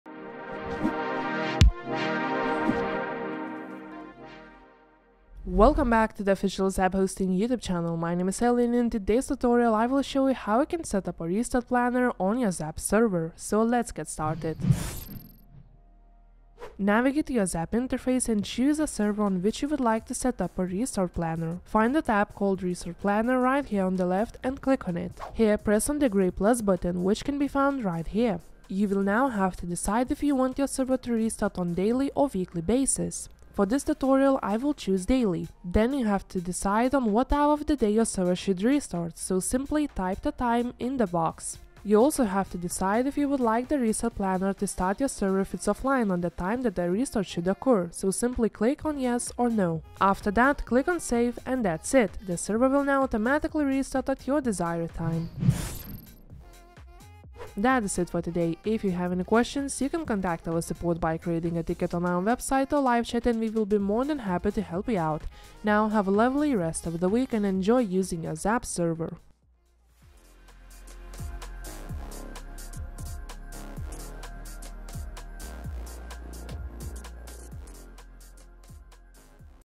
How to setup a RESTART PLANNER for your Server! | 2025
TIMESTAMPS:
0:00 Intro
0:15 Creating restart planner
0:55 Outro